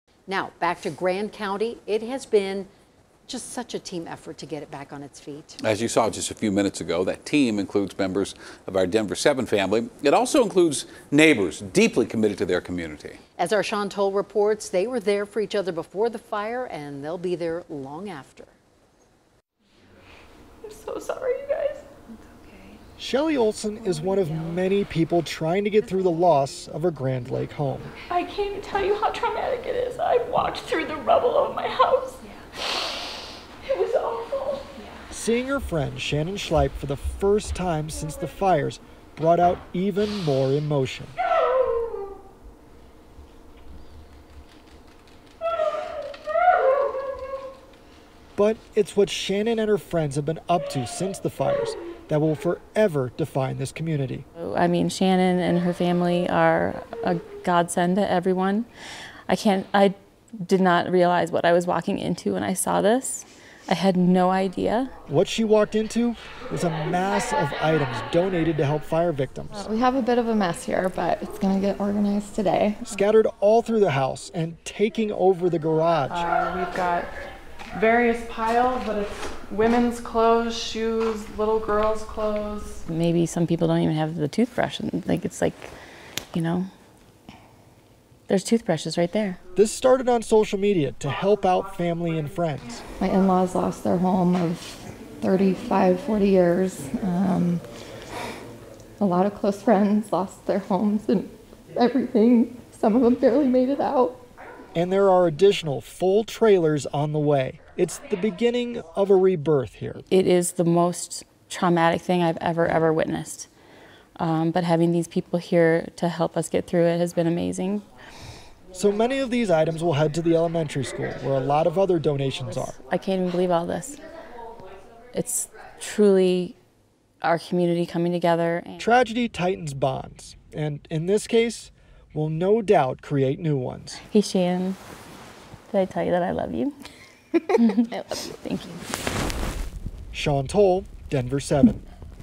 Group's efforts to help fire victims grows into something bigger
A group of friends small donation drive for Grand Lake victims of the East Troublesome Fire has turned into something much larger, thanks to the kindness of the community.